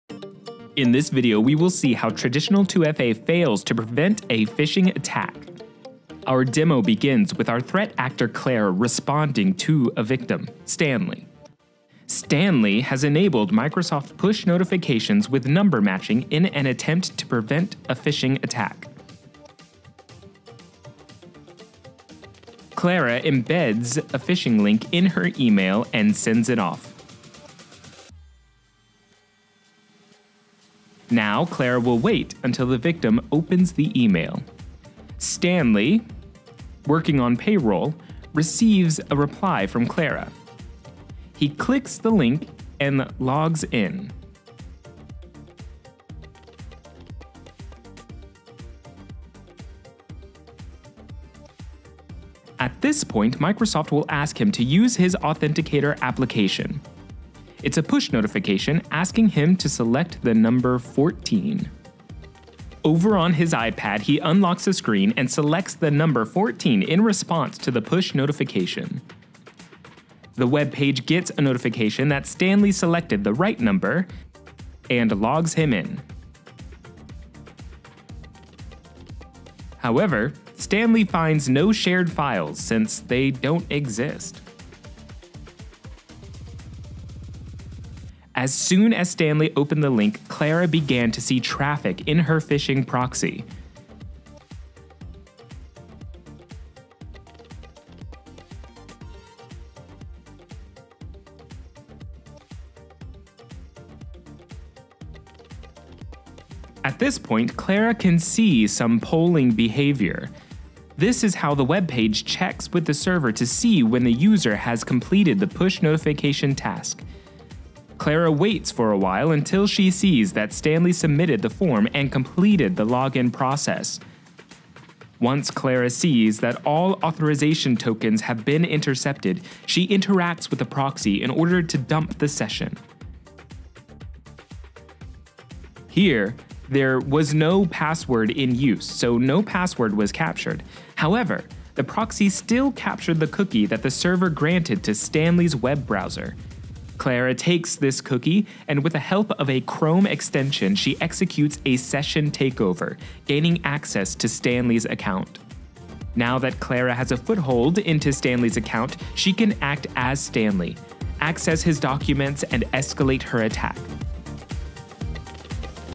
Hacking Microsoft in Real Time: Number Matching
Is your trust in Microsoft's Number Matching Multi-Factor Authentication (MFA) misplaced? Prepare to have your eyes opened in this eye-opening video where we expose the limitations of traditional 2FA in the face of a phishing attack.

In this revealing demonstration, we cover:

🕵️‍♂️ Phishing Attack: Witness how a threat actor, Clara, exploits a Microsoft user, Stanley, who believes his number matching MFA is an impenetrable shield against phishing attacks.

💻 The Bait: Clara sets the trap with a phishing link embedded in an email and waits for Stanley to take the bait.

📲 Authentication Process: Stanley receives a push notification and is asked to select the number 14 on his Authenticator application. Watch as he unknowingly complies.

🔒 False Security: The phishing webpage receives the green light, but Stanley finds no shared files - they were never there.

🕵️‍♀️ Phishing Proxy: Clara's phishing proxy comes into play, intercepting authorization tokens and tracking Stanley's every move.

📈 Hacking the Session: Clara seizes control, exploiting the compromised session, and securing the coveted cookie, even without capturing a password.

This video serves as a stark reminder that traditional MFA is vulnerable.